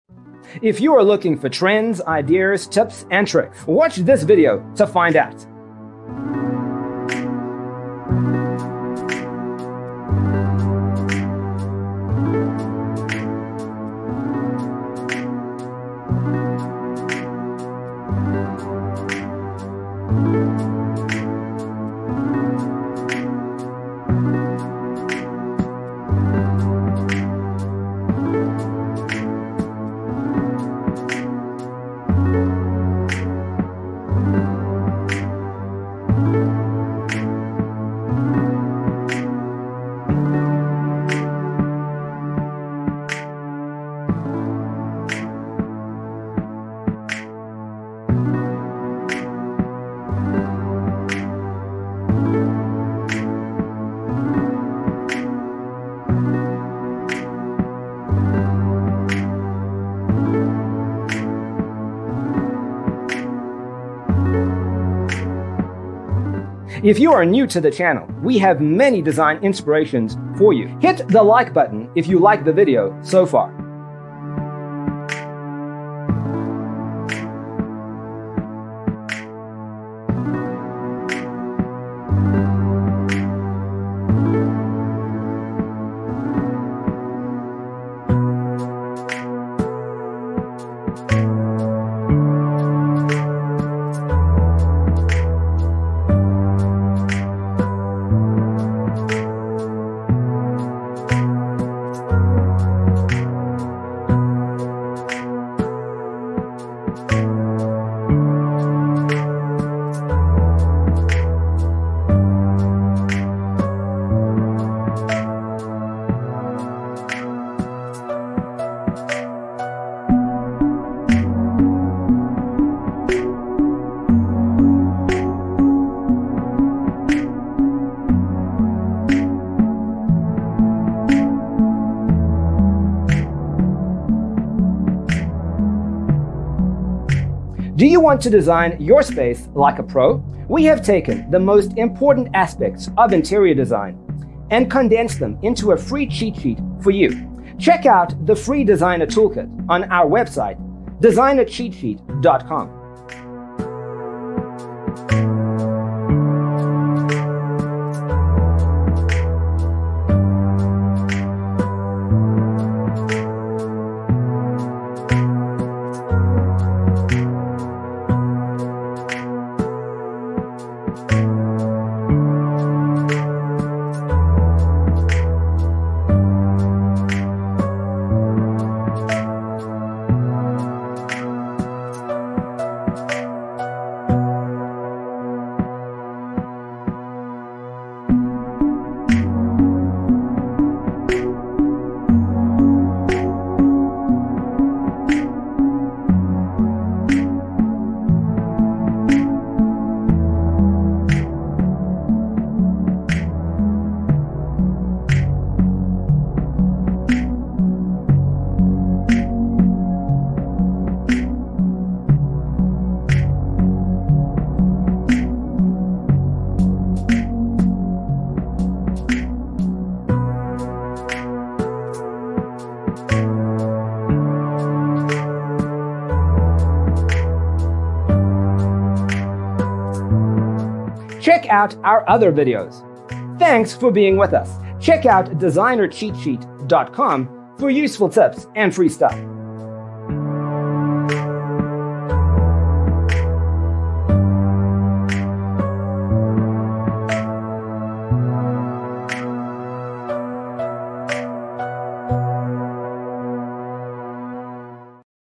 75 Home Bar With Stone Slab Backsplash Design Ideas You'll Love 🔴
0:00:00 - Intro
0:00:12 - Home Bar With Stone Slab Backsplash Design Ideas You'll Love: Inspirations 1 to 25
0:01:18 - Home Bar With Stone Slab Backsplash Design Ideas You'll Love: Inspirations 26 to 50
0:02:36 - Home Bar With Stone Slab Backsplash Design Ideas You'll Love: Inspirations 51 to 75